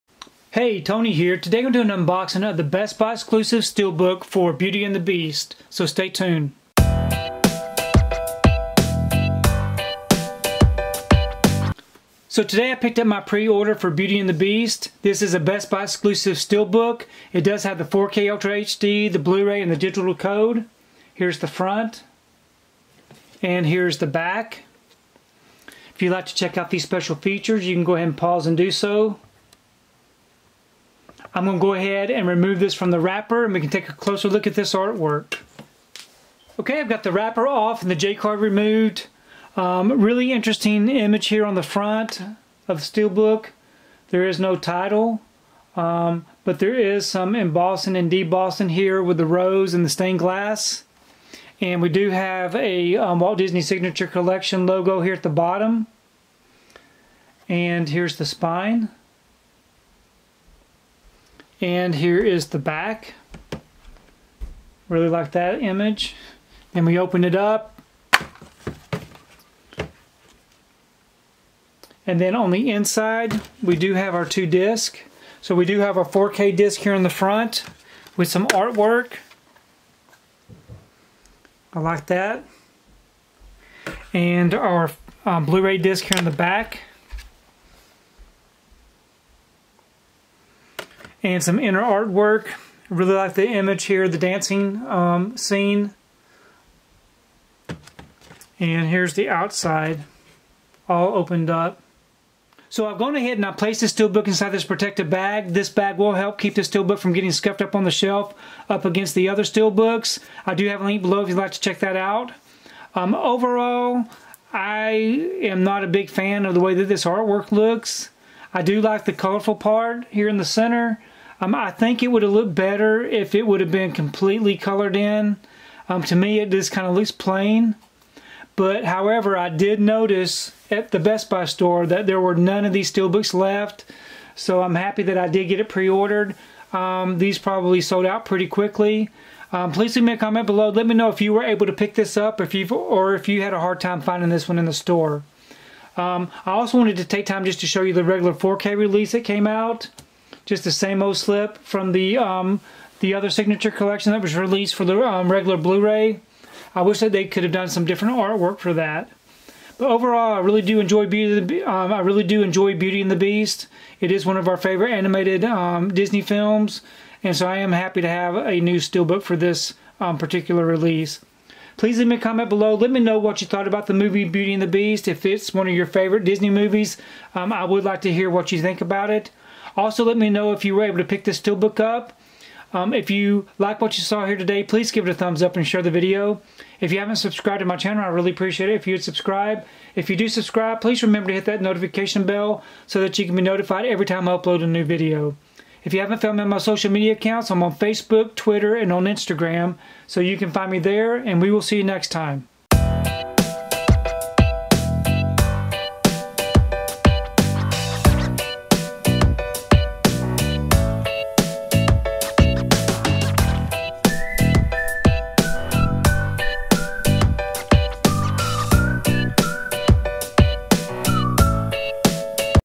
Beauty and the Beast Best Buy Exclusive 4K Ultra HD Blu-ray Steelbook Unboxing
Check out my unboxing of the Disney animated Beauty and the Beast Best Buy Exclusive Steelbook on 4K Ultra HD Blu-ray!

Purchase your own 4K Blu-ray

These are the bags I use to protect my Steelbooks and Slipcovers: